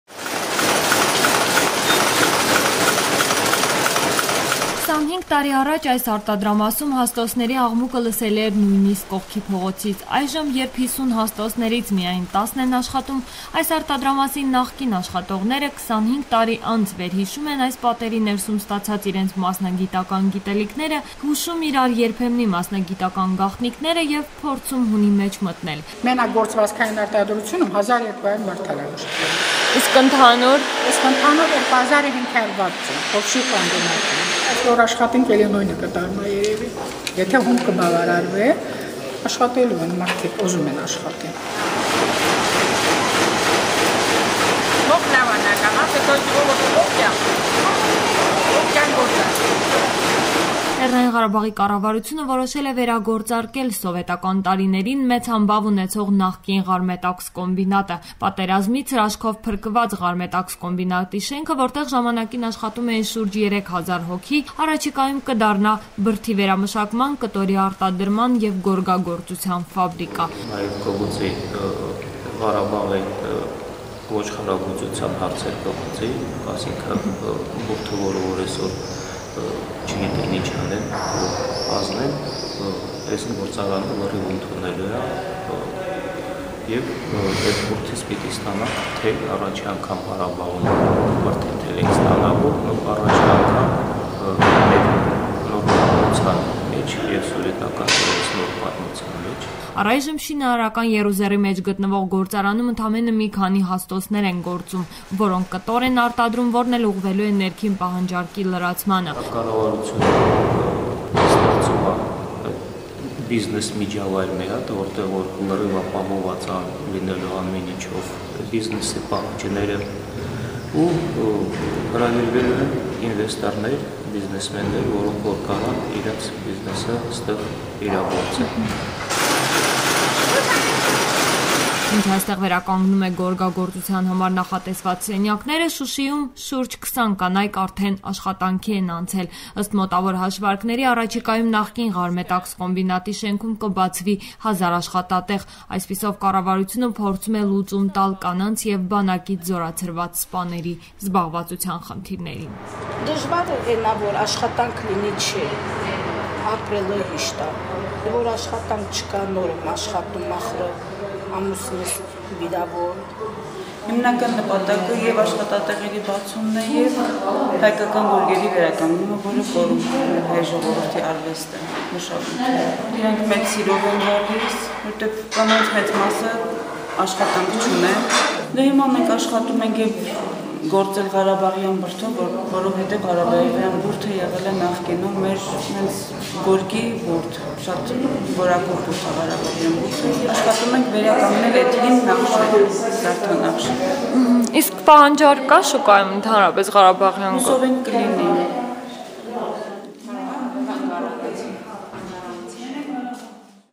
Ղարաբաղում նոր աշխատատեղեր են բացվում
ԼՂ կառավարությունը որոշել է վերագործարկել խորհրդային տարիներին մեծ համբավ ունեցող նախկին «Ղարմետաքսկոմբինատը»: Պատերազմից հրաշքով փրկված շենքը, որտեղ ժամանակին աշխատում էին շուրջ երեք հազար հոգի, առաջիկայում կդառնա բրդի վերամշակման, կտորի արտադրման եւ գորգագործության ֆաբրիկա: